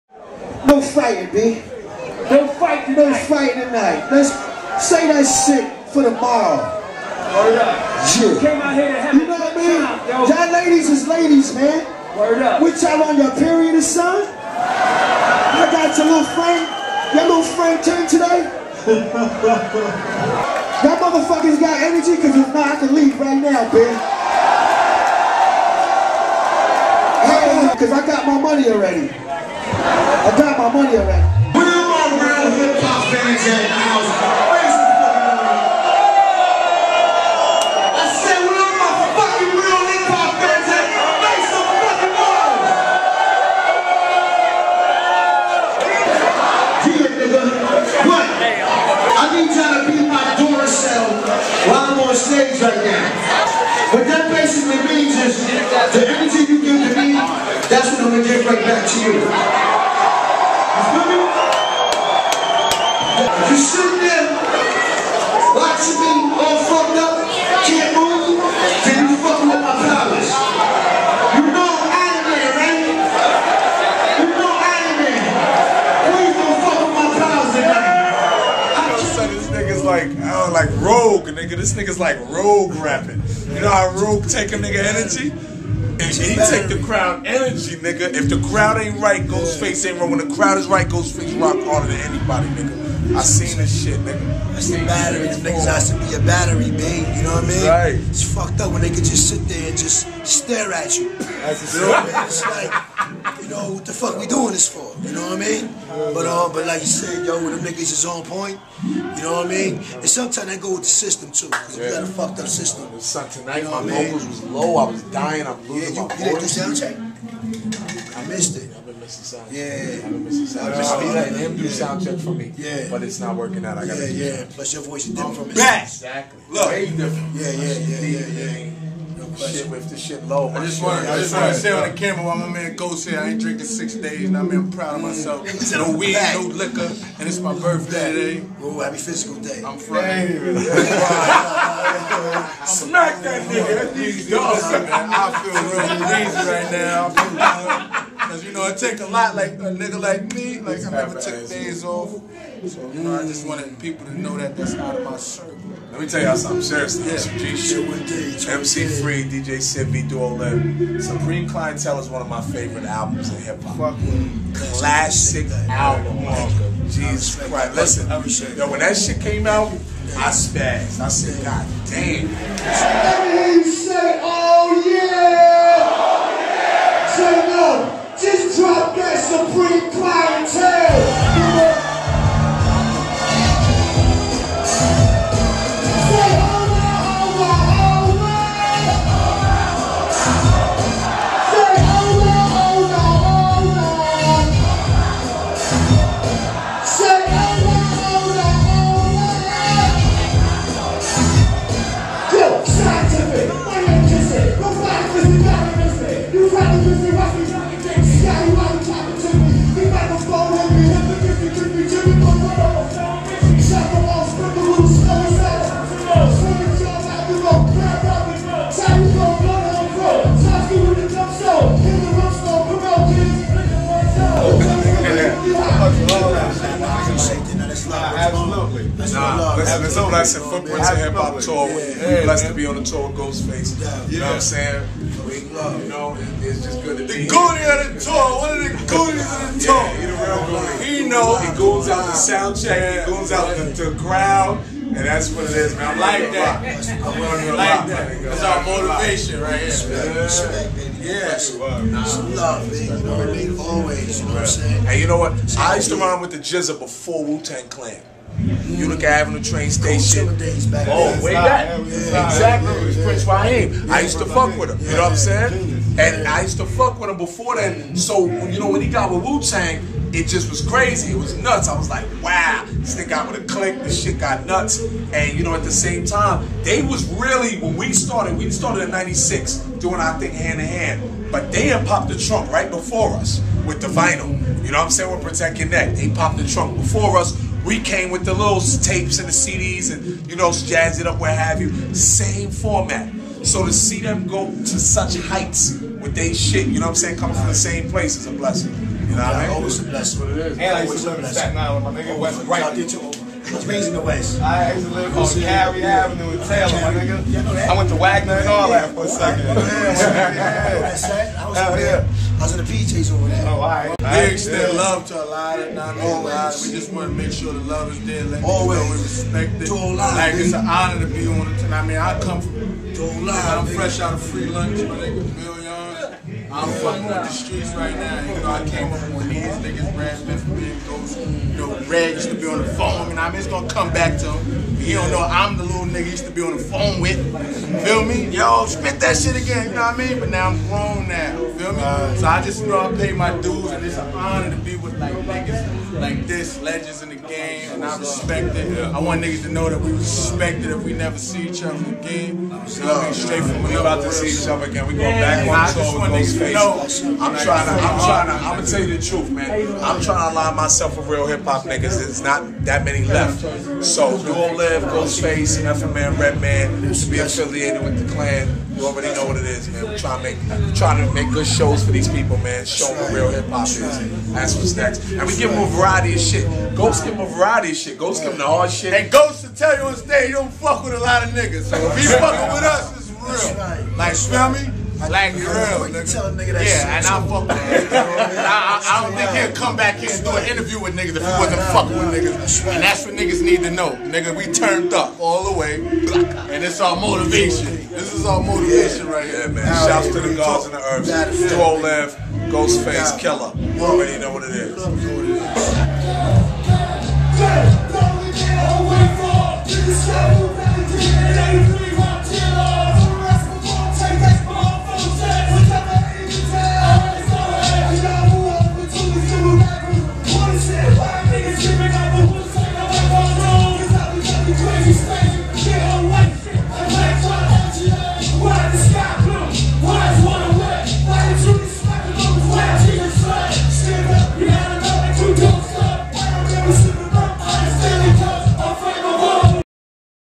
Duo Live & Ghostface (Footprints Tour)
Duo Live and Billionz kickin it with ghostface on the footprints tour